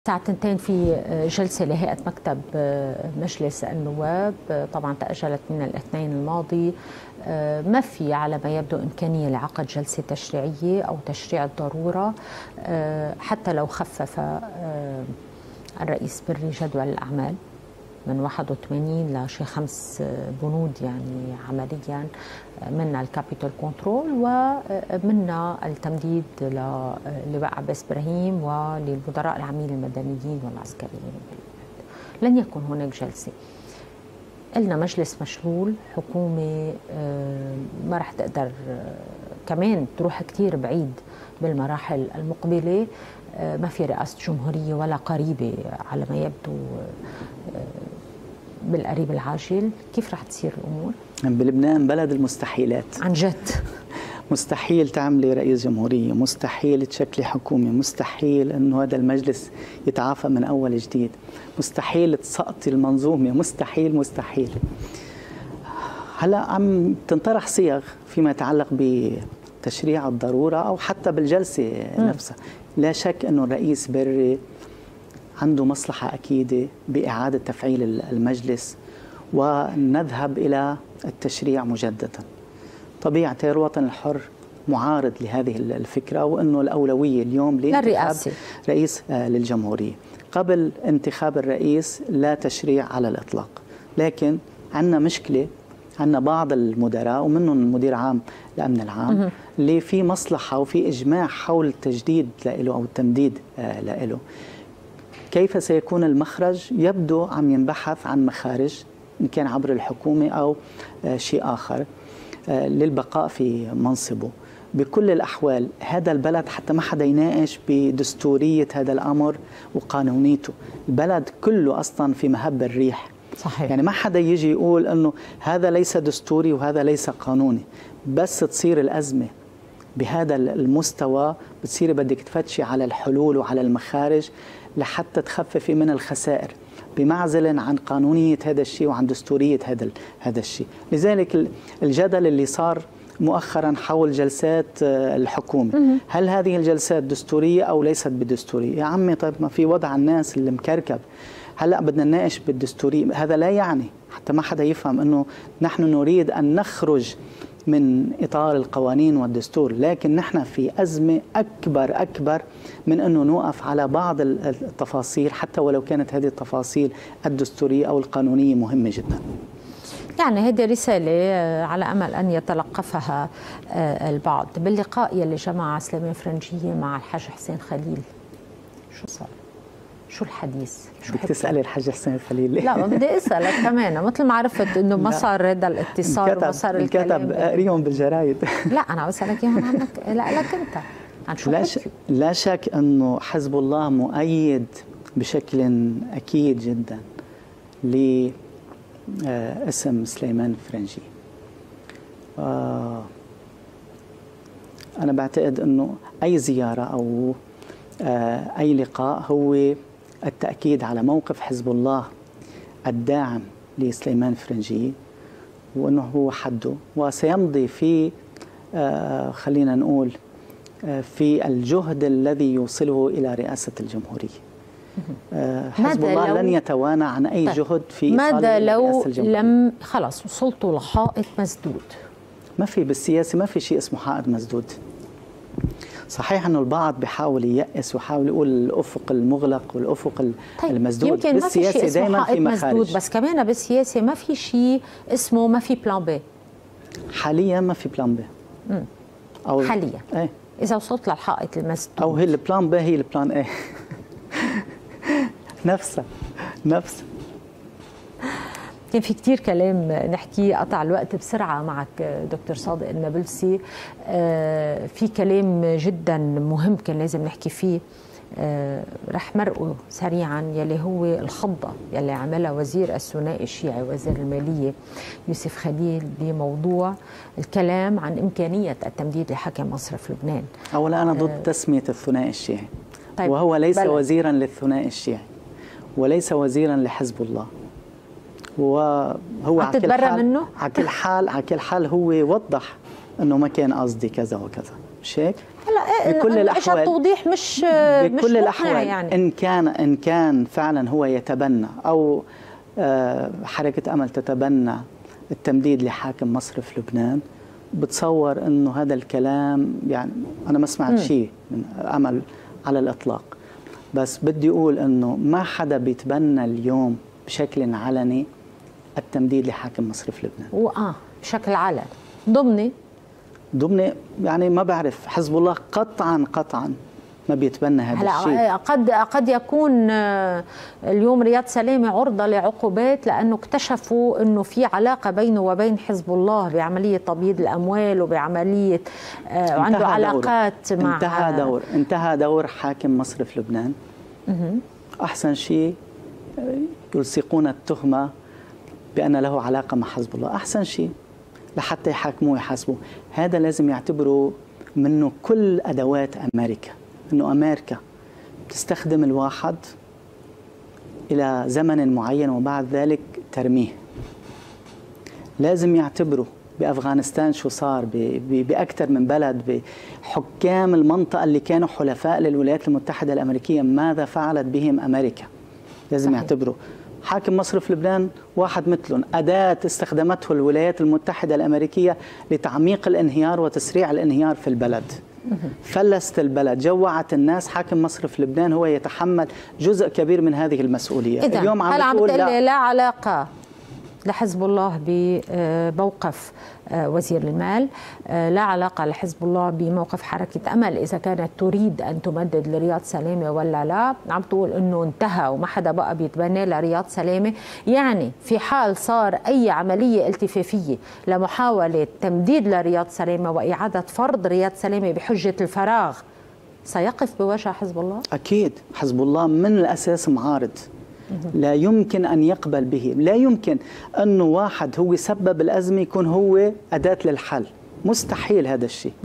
صادق النابلسي يكشف كواليس لقاء فرنجية والخليل : هذا الشخص انتهى دوره وهذه مصلحة بري !
أشار أستاذ العلوم السياسية في الجامعة اللبنانية الشيخ الدكتور صادق النابلسي، خلال مقابلة عبر "الحدث"، إلى أن "هذا بلد المستحيلات.. ورئيس مجلس النواب نبيه بري لديه مصلحة اكيدة بإعادة تفعيل المجلس النيابي والذهاب للتشريع مجددا، ولكن التيار الوطني الحر معارض للفكرة". وأضاف: "لدينا بعض المدراء منهم اللواء عباس ابراهيم، وهناك مصلحة وإجماع حول التجديد له، ويتم البحث عن مخارج للبقاء في منصبه".